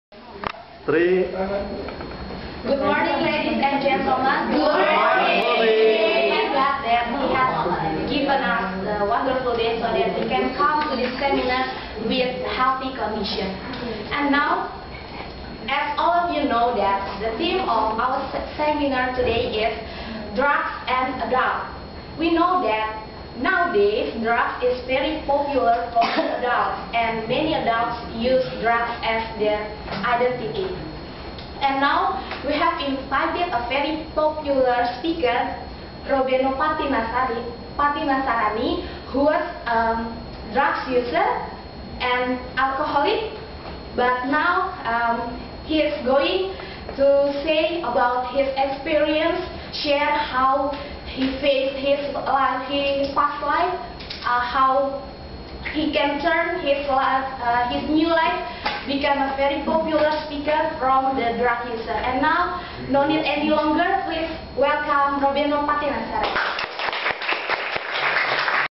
Leni P-Speech of Introduction
Leni, your speech will be interesting if you observe these three aspects of speech delivery. You should have eye contact with your audience and it has to be 3 seconds. It's not that long and it's not that short. 3 seconds are important because it makes the audience engaged with you. Next, you should improve your body language. Gesture support your speech the way spices give flavor to food. Finally, you should speak louder than this one on video. I trust you can improve next time.